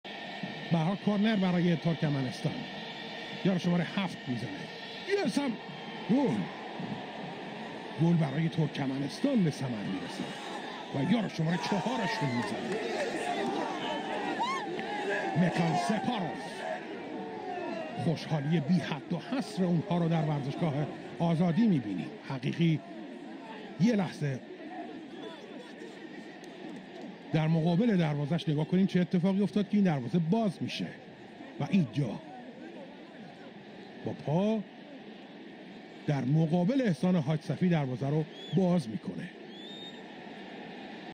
Mekan Saparow goal 62' (Iran 3-1 Turkmenistan)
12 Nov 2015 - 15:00
FIFA World Cup™ Qualifier
Azadi Stadium Tehran (IRN)